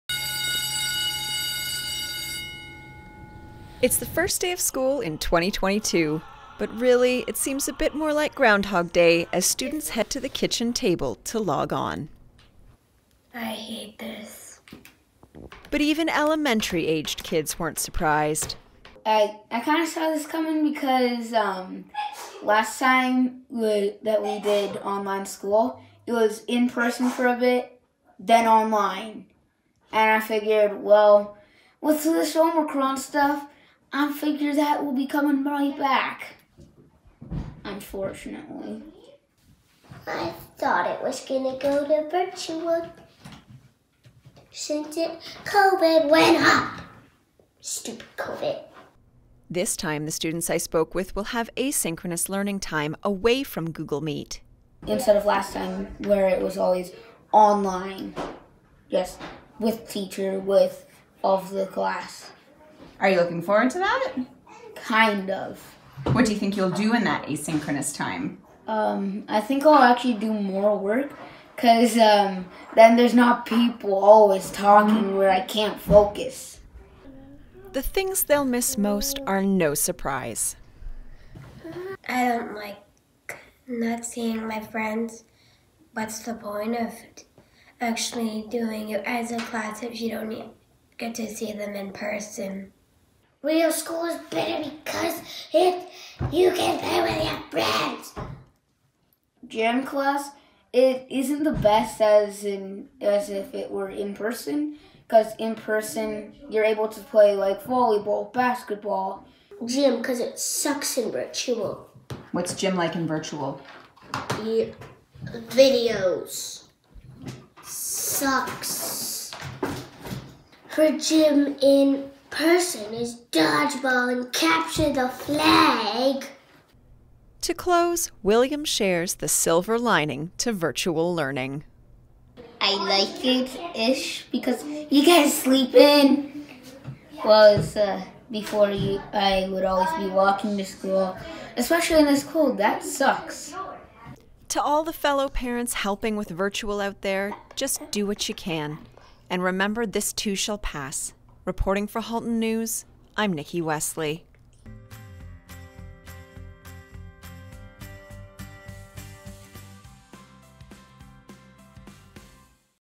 Back to the Virtual Classroom
Today was the first day of school in 2022 and we caught up a few students to get their feelings as they head back to the virtual classroom.